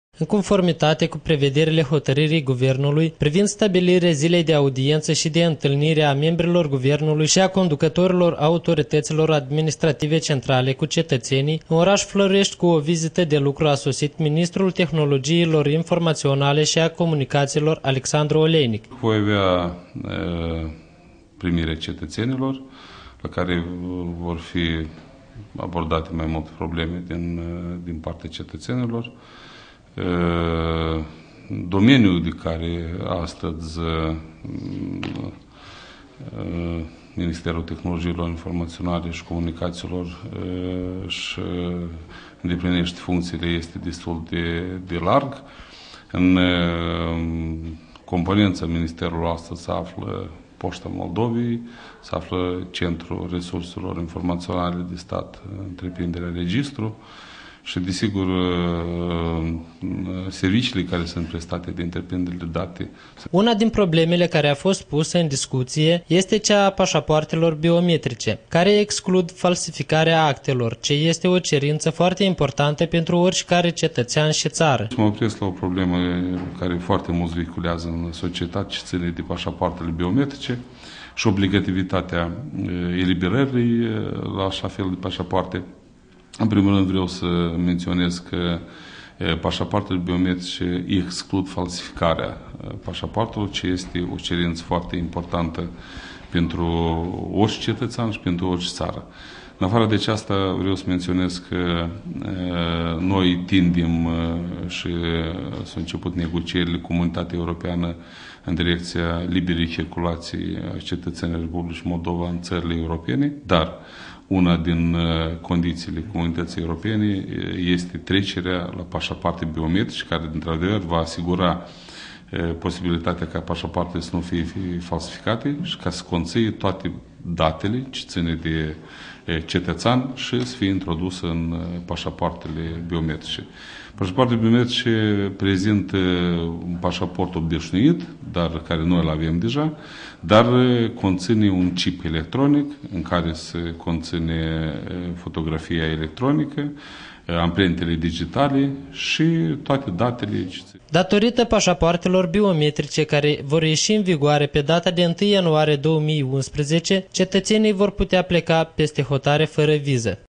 AUDIENŢĂ CU ALEXANDRU OLEINIC
LA 20 APRILIE, IN CONFORMITATE CU PREVEDERILE HOTARIRII GUVERNULUI   PRIVIND STABILIREA  ZILEI DE AUDIENTA  SI DE INTILNIRE A MEMBRILOR GUVERNULUI  SI A CONDUCATORILOR AUTORITATILOR ADMINISTRATIVE CENTRALE CU CETATENII, IN ORASUL FLORESTI , CU O VOZITA DE LUCRU  A SOSIT  MINISTRUL TEHNOLOGIILOR  INFORMATIONALE SI  A COMUNICATIILOR , ALEXANDRU MELNIC. UNA DIN PROBLEMELE CARE A FOST  PUSA IN DISCUTIE  ESTE CEA A PASAPOARTELOR  BIOMETRICE , CARE EXCLUD FALSIFICAREA  ACTELOR  ESTE O CERINTA  FOARTE IMPORTANTA PENTRU ORISICARE CETATEAN  SI TARA. DATORITA PASAPOARTELOR BIOMETRICE, CARE VOR IESI IN VIGOARE PE DATA DE 1 IANUARIE 2011, CETATENII VOR PUTEA PLECA PESTE HOTARE FARA VIZE.